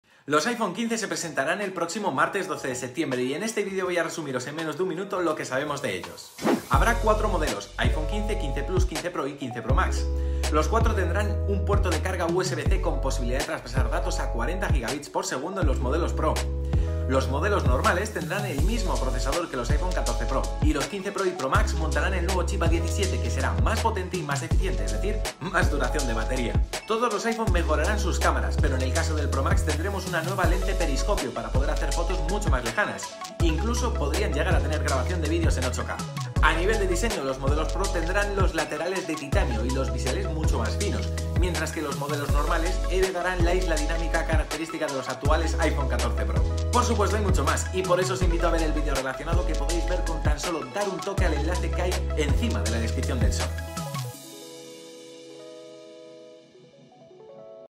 Así son los nuevos iPhone 15 (en menos de 1 minuto)
Los iPhone 15 se presentarán el próximo martes 12 de septiembre y en este video voy resumiros en menos de 1 minuto lo que sabemos de ellos.

⭐️ Hazte suscriptor de La Manzana Mordida+ y disfruta de sorteos, acceso a nuestro Telegram, un podcasts exclusivo y mucho más